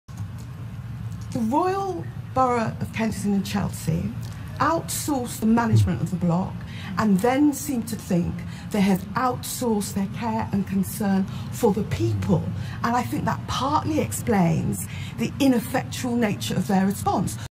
Those responsible for the Grenfell Tower fire must be held to account for their actions
Diane Abbott MP calling for action against all those responsible of the Grenfell Tower fire, particularly Kensington and Chelsea's council, that have been warned for years by residents of the terrible consequences that could occur as their safety was not guaranteed.